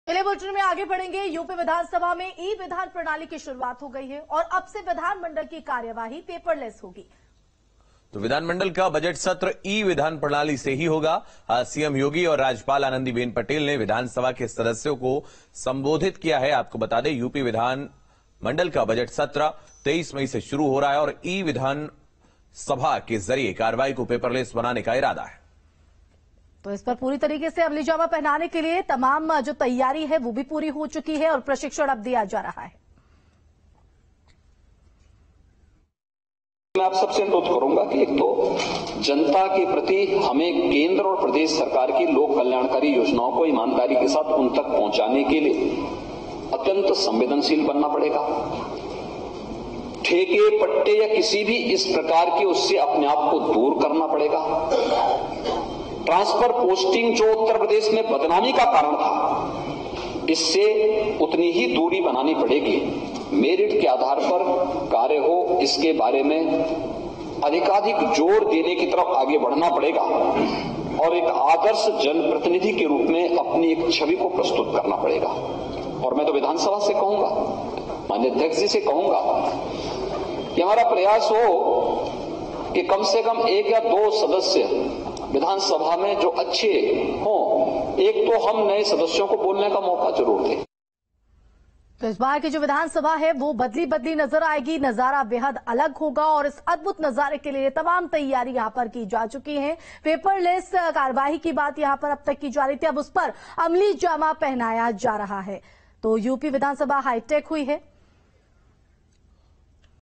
CM Yogi: जातिवाद और ट्रांसफर पोस्टिंग को लेकर सीएम योगी ने कही बड़ी बात
News State Apps डाउनलोड करें:
Download Android and iOS Apps for more updates: